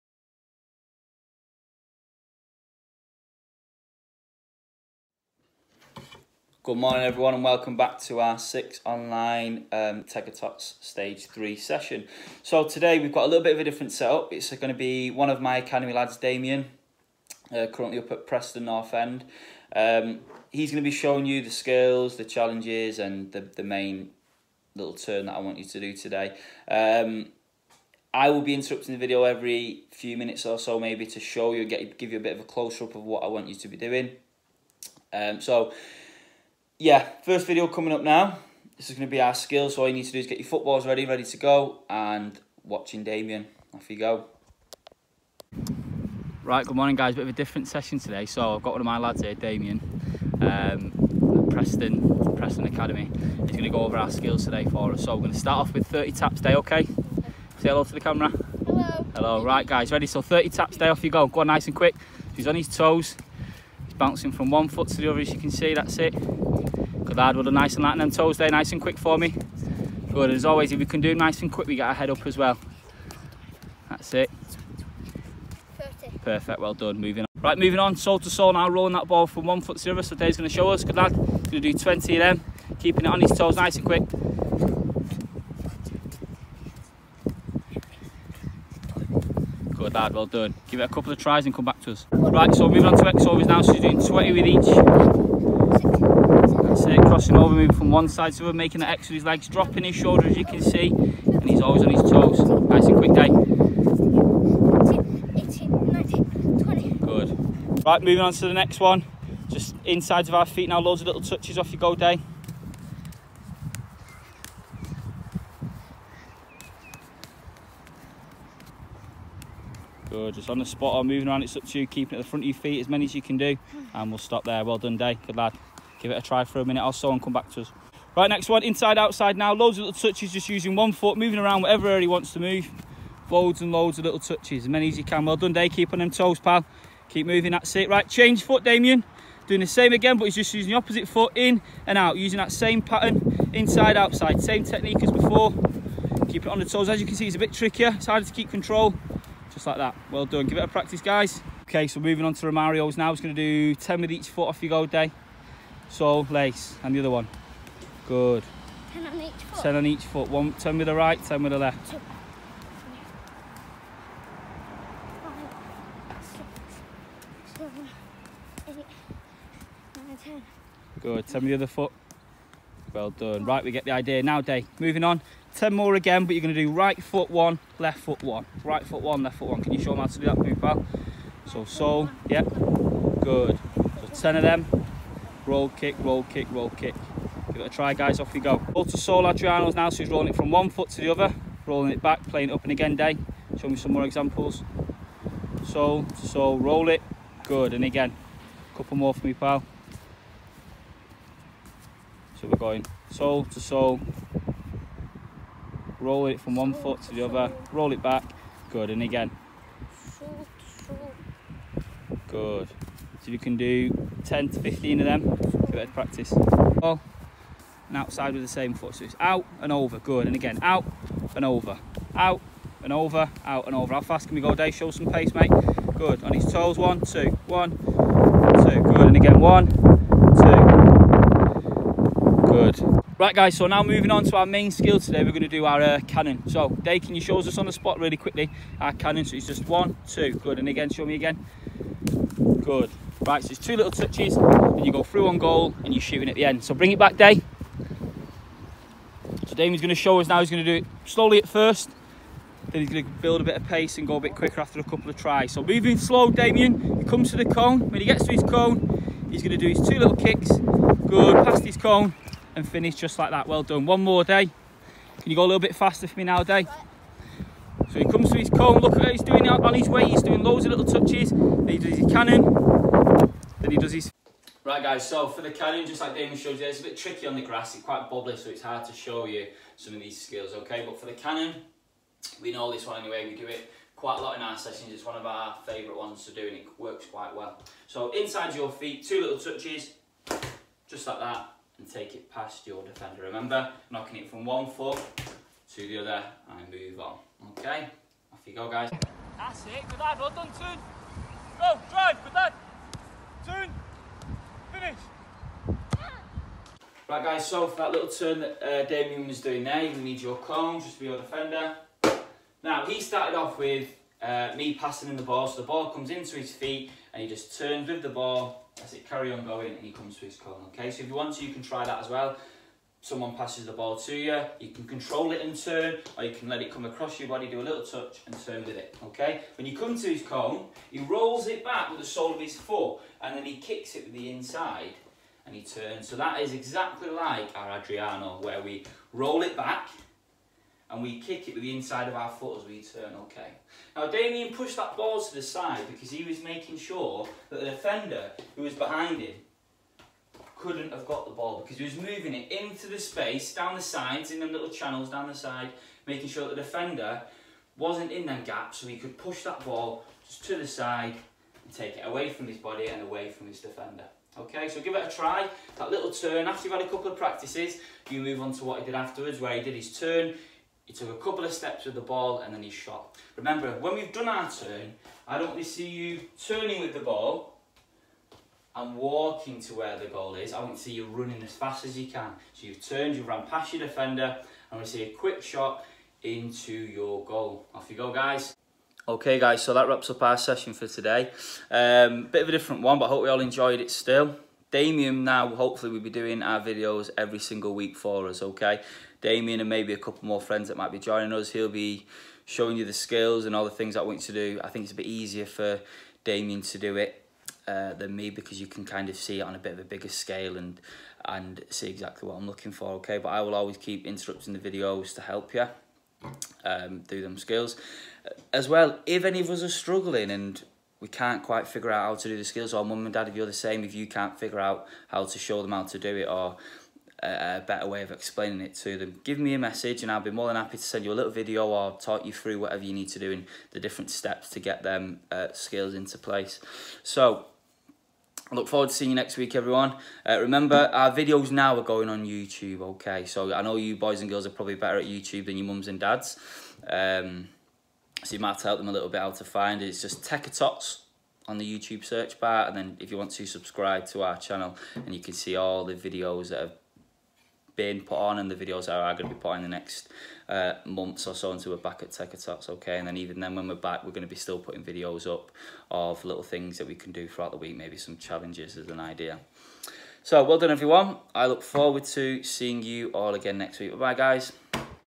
Tekker Tots stage 3 Week 6
TEKKER TOTS Stage 3 - Week 6 - 9:55am class

Hi Everyone, here is our week six session, I hope you enjoy! Remember to send those pictures and videos for us to upload 😁📸